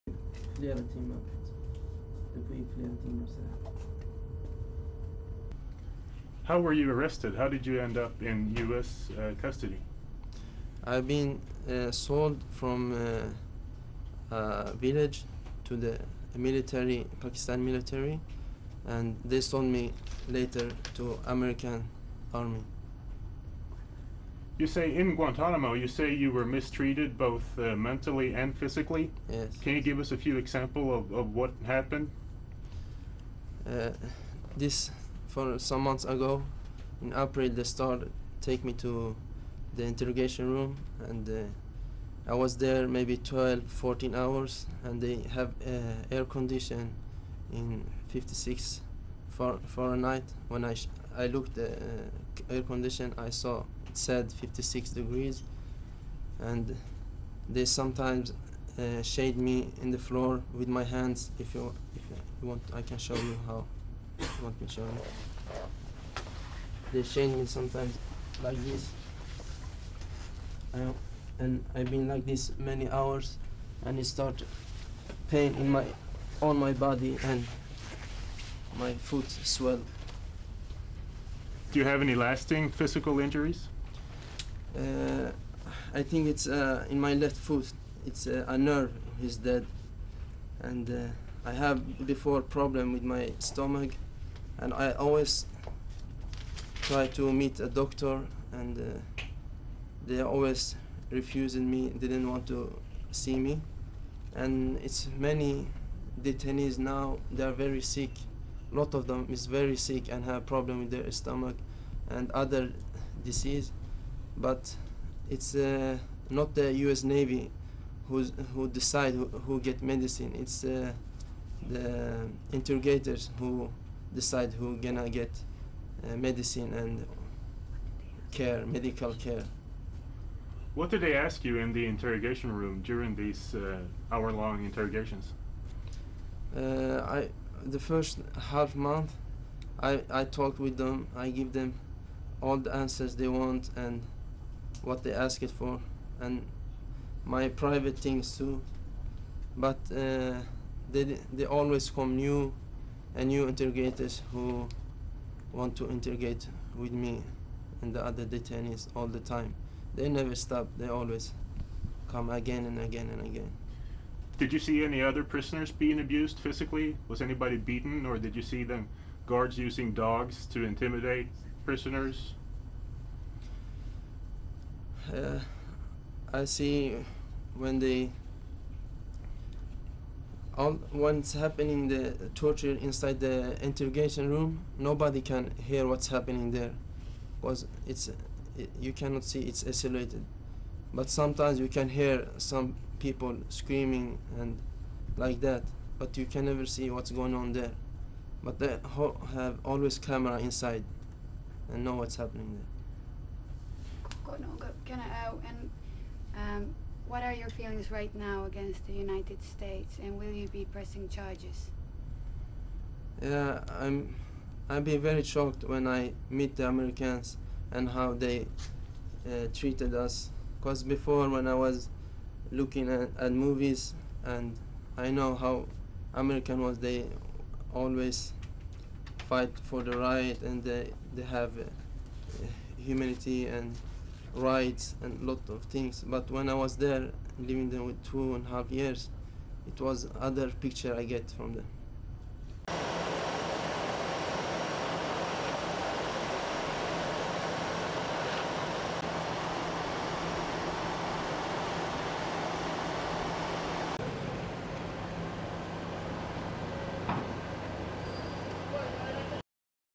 Former Guantanamo detainee describes alleged abuse
1. Mehdi-Muhammed Ghezali showing how he was allegedly chained up by US jailers
2. SOUNDBITE (English) Mehdi-Muhammed Ghezali, former Guantanamo detainee:
"(Q: How were you arrested, how did you end up in US custody?)
I'd been sold from village to the military, Pakistan military and they sold me later to American army.
(Q: You say in Guantanamo you were mistreated both mentally and physically?)
Yes.
(Q: Could you give us a few examples of what happened?)
This for some months ago and (inaudible) take me to the interrogation room and I was there maybe 12, 14 hours and they have air conditioning at 56 fahrenheit, when I looked at air conditioning I saw it said 56 degrees, and they sometimes chained me in the floor in my hands, if you want I can show you how...  They chained me sometimes like this many hours and it start pain on my body and my foot swell.
(Q: Do you have any lasting physical injuries?)
I think it's in my left foot, a nerve that's dead and I have before problem with my stomach, and I always try to meet a doctor and they are always refusing me and didn't want to see me and it's many detainees now, they is very sick, a lot of them are very sick and have problem with their stomach and other disease, but it's not the US navy who decides who gets medicine, it's the interrogators who are going to get medicine and care, medical care.
(Q: What did they ask you in the interrogation room during these hour long interrogations?)
The first half month I talked with them, I give them all the answers they want and what they asked for and my private things too, but they always come, new interrogators who want to interrogate with me and the other detainees all the time. They never stop, they always come again and again and again.
(Q: Did you see any other prisoners being abused physically, was anybody beaten or did you see the guards using dogs to intimidate prisoners?)
I see when they... all what's happening in torture, inside interrogation room, nobody can hear what's happening there, you cannot see, it's isolated. But sometimes you can hear some people screaming like that but you can never see what's going on there. But they have always camera inside and know what's happening there.
(Q: What are your feelings now against the United States and will you be pressing charges?) I'm being very shocked when I meet the Americans and how they treated us, because before when I was looking at movies and I know how America was always fighting for the rights and they have humanity and rights and a lot of things, but when I was there (Guantanamo) for two-and-a-half years I got a different picture from them."
3. Various exteriors of television vans

STORYLINE:

A Swede detained by the United States for more than two years at Guantanamo naval base in Cuba, on Friday denied any links to al-Qaida and repeated claims he was physically and mentally tortured by US jailers and interrogators.

Mehdi-Muhammed Ghezali told reporters he didn't see other prisoners tortured at the US military prison "but you could hear people screaming sometimes."

The Pentagon dismissed his claims.

Ghezali - born in Sweden to a Finnish mother and Algerian father - was reportedly part of a group of 156 suspected al-Qaida fighters arrested in 2001 by Pakistani authorities while fleeing Afghanistan's Tora Bora mountains.

Ghezali, 25, was released by US authorities last week and flown home to Sweden.

Ghezali repeated claims to Swedish media earlier this week that he was subjected to torture during his imprisonment at Guantanamo.